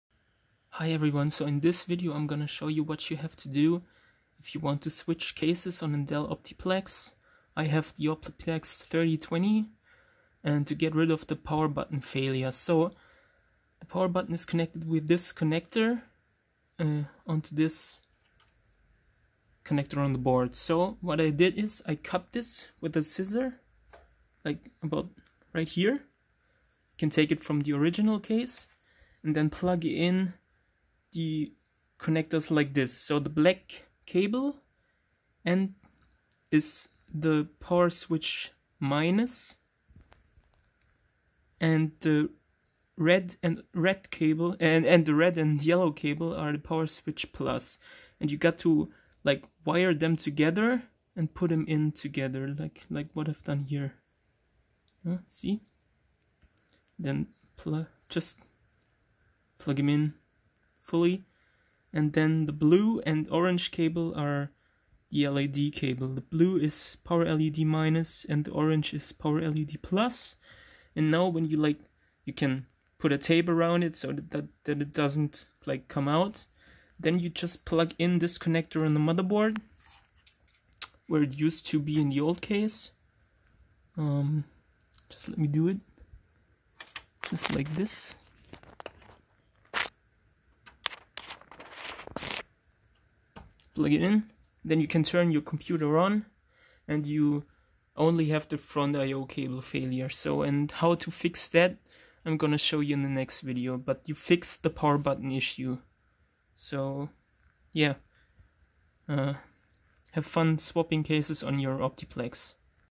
Swap Case on Dell Optiplex 3020 - Power Button and front i/o cable Failure; Connection+Solution 1/2
Hello everyone,
in this Video i am going to show you how to get rid of the Power Button Connection Failure during the boot process of a Dell Optiplex 3020 after moving it into a new case.
It is a very simple solution. All you need are scissors and the willingness to sacrifice your old cables from the original case it shipped in.